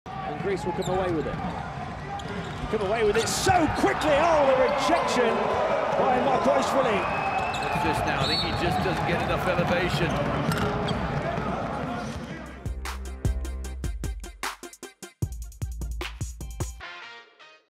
Markoishvili's Huge Rejection! - EuroBasket 2015
Manuchar Markoishvili with a massive block on Papanikolaou!